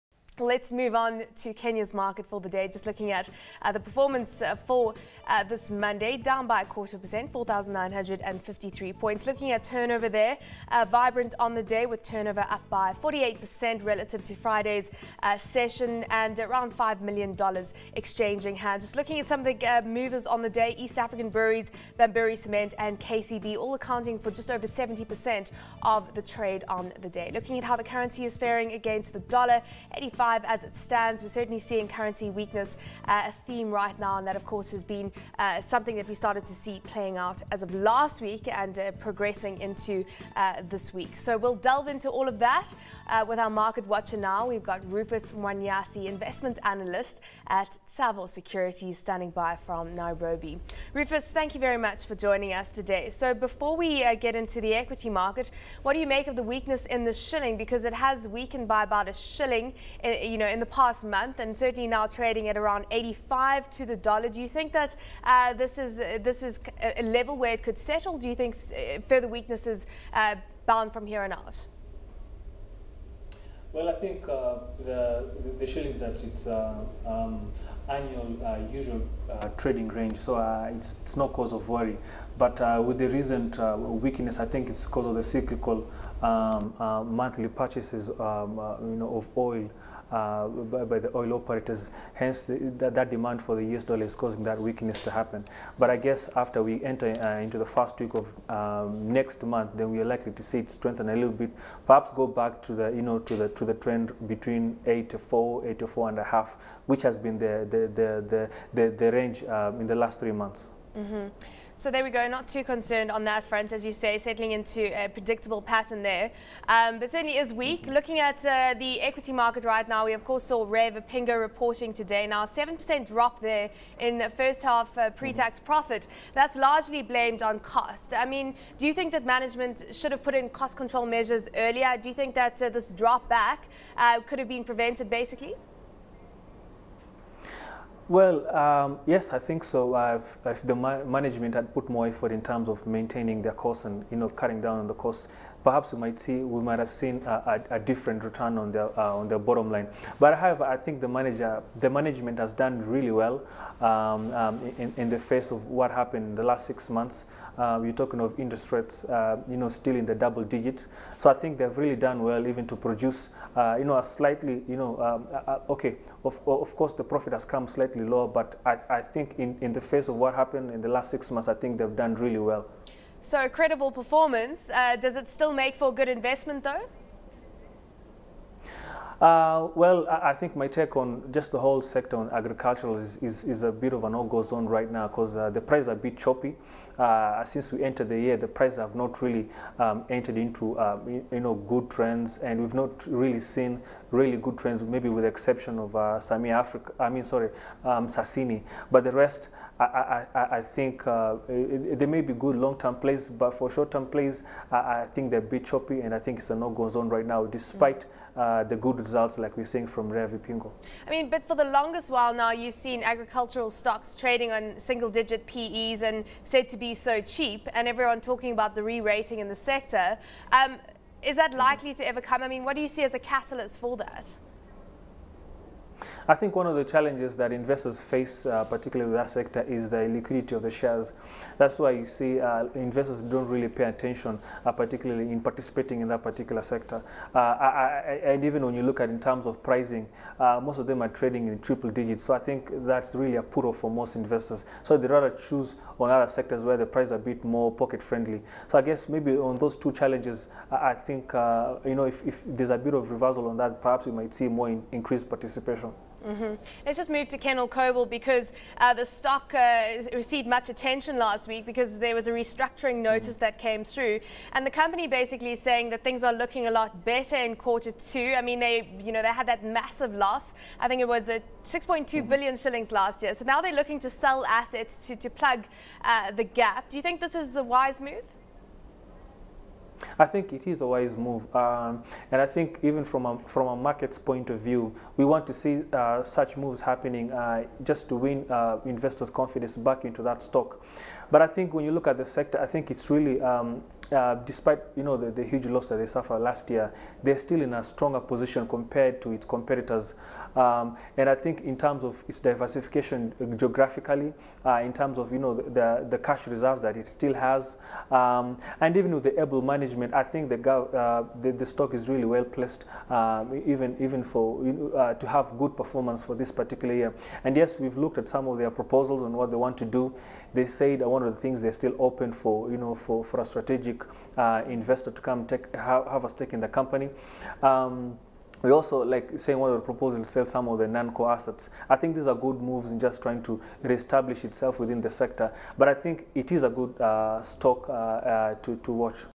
27 May - Kenya Markets Wrap with Rufus Manyasi
ABN's Samantha Loring speaks with Rufus Manyasi, Investment Analyst at Tsavo Securities,  looking at the latest financial news driving the Kenyan market.